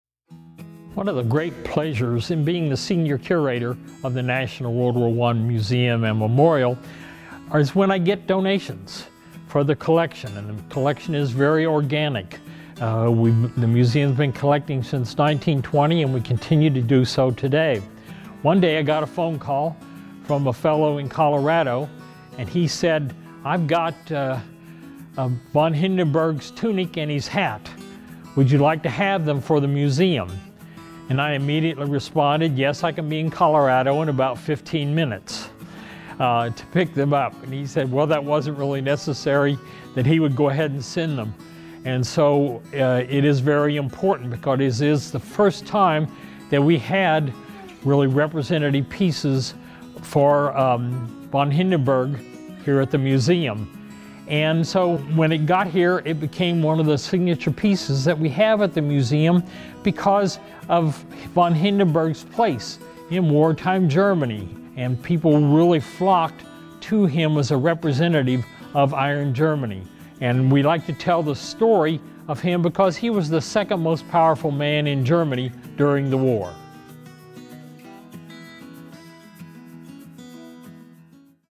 Museum Minute: Von Hindenburg Display
Senior Curator Doran Cart shares how a tunic and cap worn by Von Hindenburg became a part of the Museum's collection

For more information about the National WWI Museum and Memorial